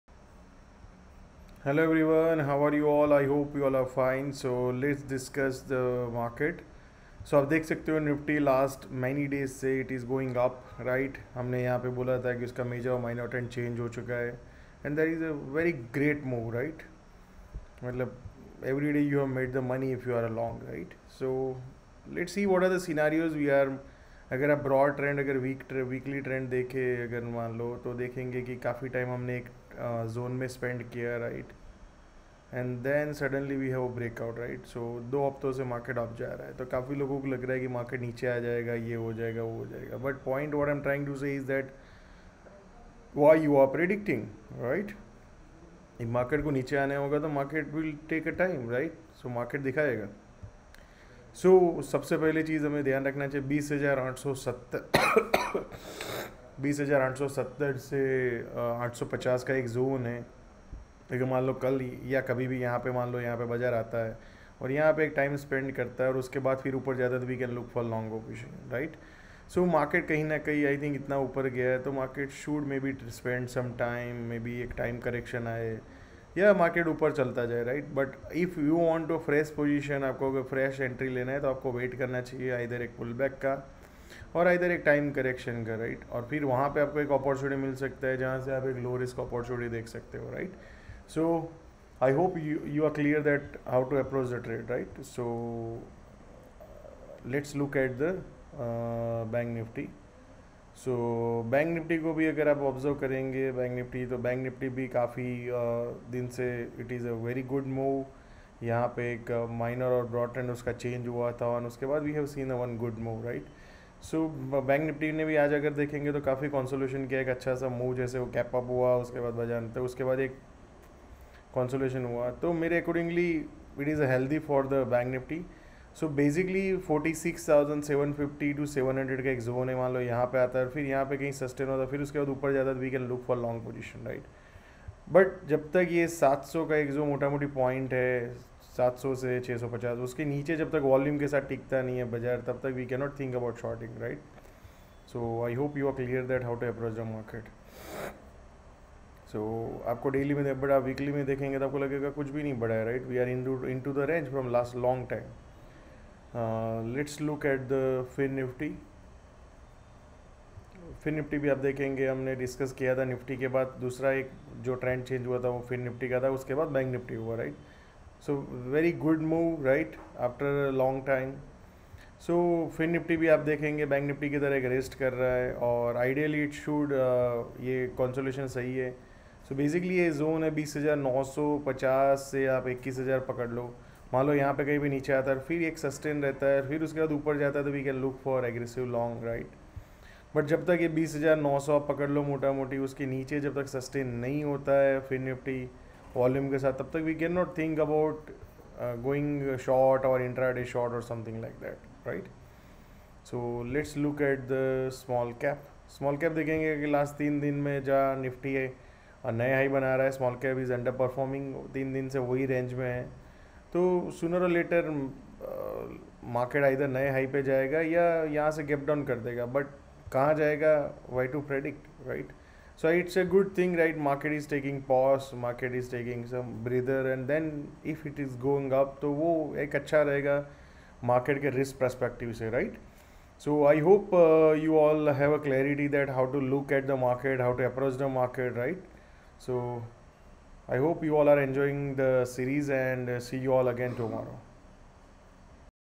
Nifty & Banknifty Analysis | Will Market Correct From Here? | For 7 Dec. 2023 | Episode - 140
✔️ Open a Free Demat Account in Zerodha from the

✔️ Join Niftymillionaire Community [ Call ] : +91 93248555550

✔️ Join Telegram Channel for Daily Market Updates & Inside Info.